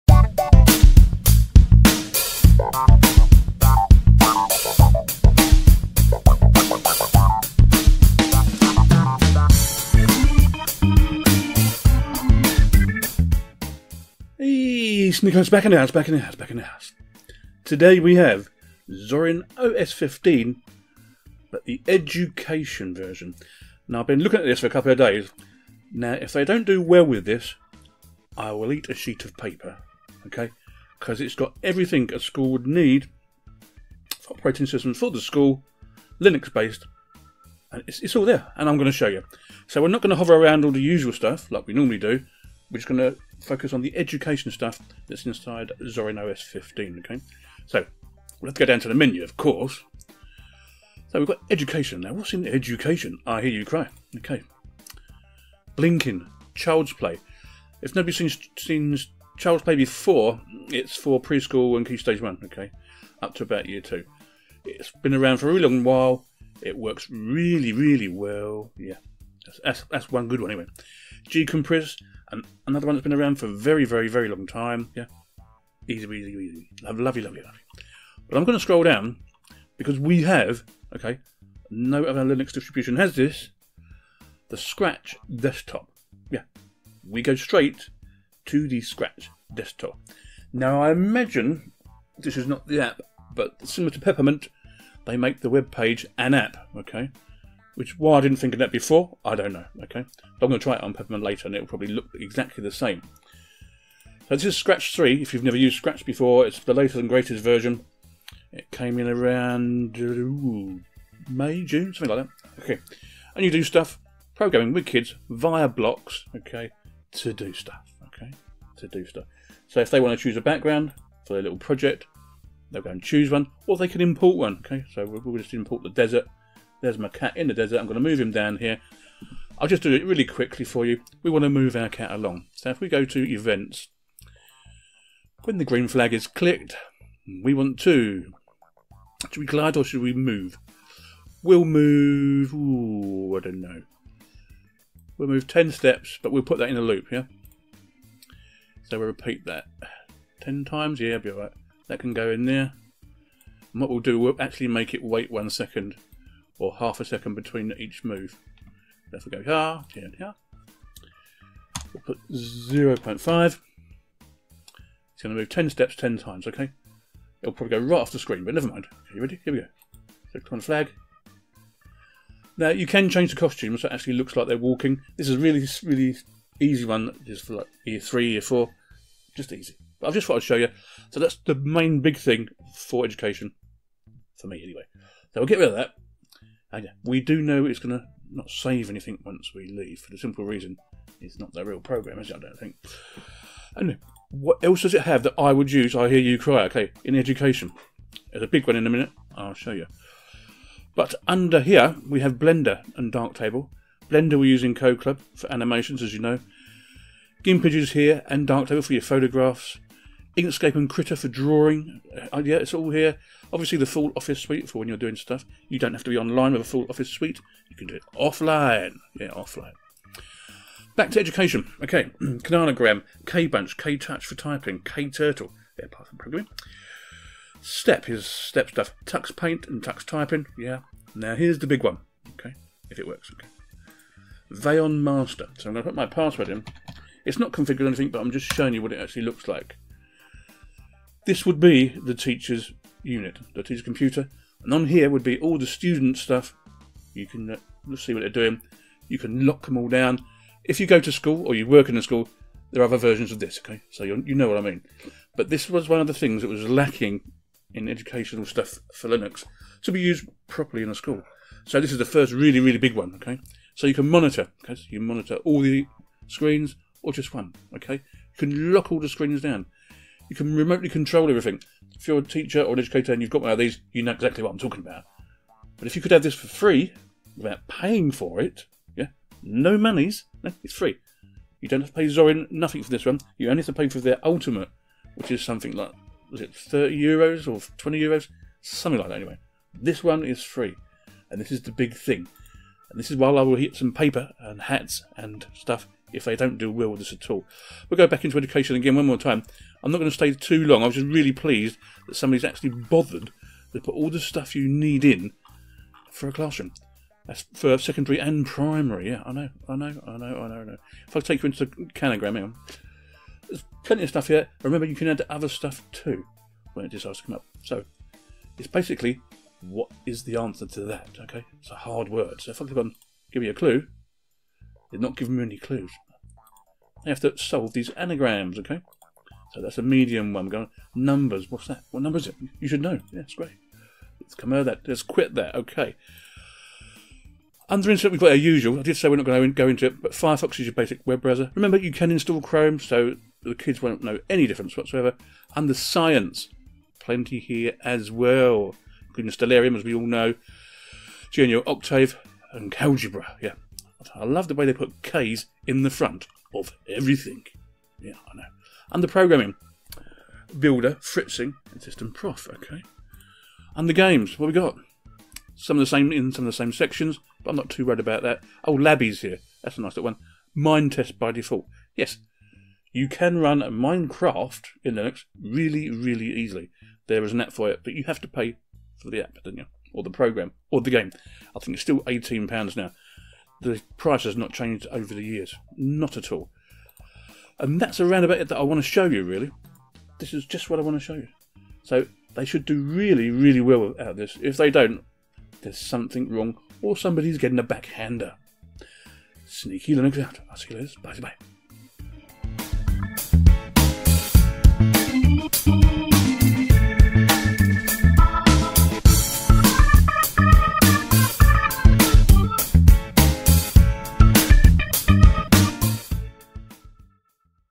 Zorin OS 15 "Education Edition"..At Last A Real Solution.
Hi chaps and chapesses, this time we are going to have a quick look at Zorin OS 15 education, and to be honest it has been a long time coming and if they don't do well with this one there must be something wrong as for schools with no cash this is a no brainer.
It even has included the scratch 3 desktop but this is more of an ice implementation to be honest but it does work.